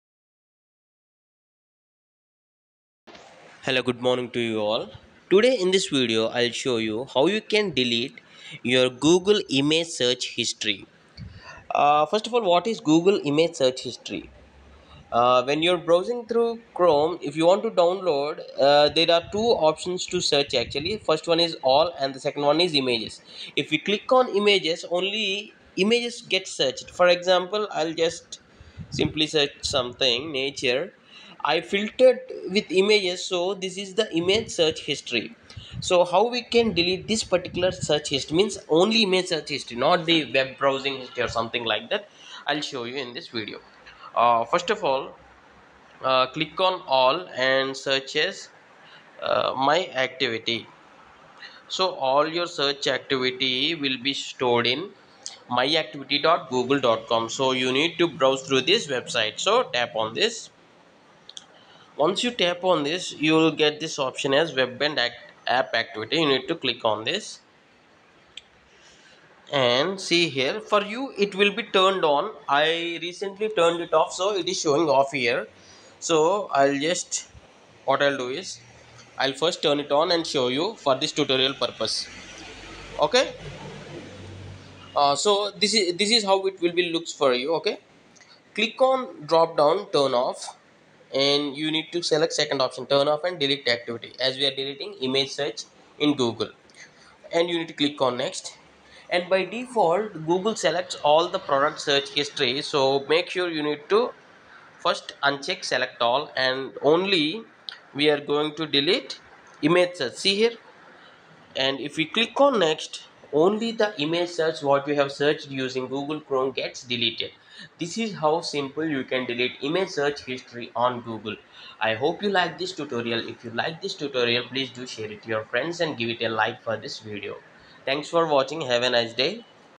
How to Delete Google Image Search History
If you want to delete all Google image search history what you have searched before we can delete in the above shown way.

In the above video tutorial there are easy steps to delete Google image search history completely.